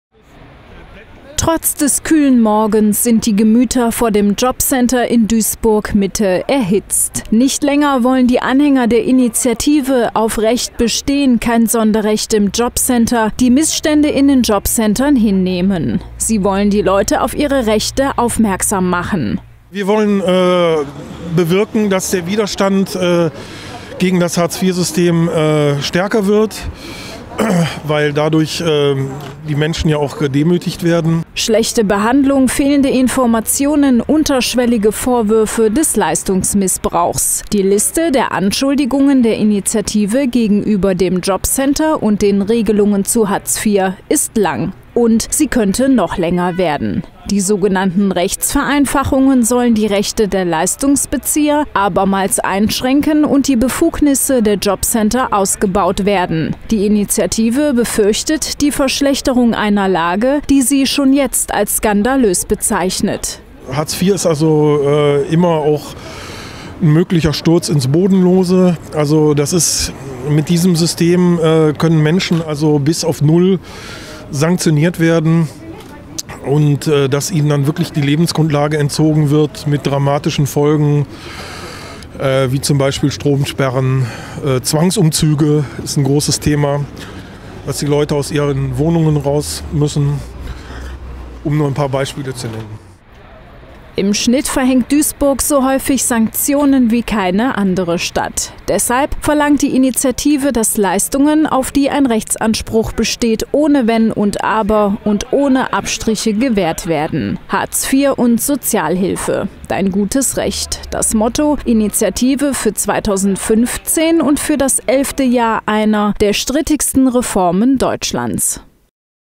Bürgerinitiative demonstriert vor Duisburger jobcenter
„AufRecht bestehen - Kein Sonderrecht im jobcenter“, so lautet der Name der Initiative, die heute vor dem Duisburger jobcenter für ein wenig Trubel gesorgt hat. Dabei wollten sie nicht nur auf das 10 Jährige Bestehen von Hartz IV aufmerksam machen, sondern auch auf die drohende Rechtsvereinfachung. Die bringt vor allem den Ausbau der Befugnisse des jobcenters mit sich. Eine Tatsache, die man nicht so einfach auf sich sitzen lassen möchte.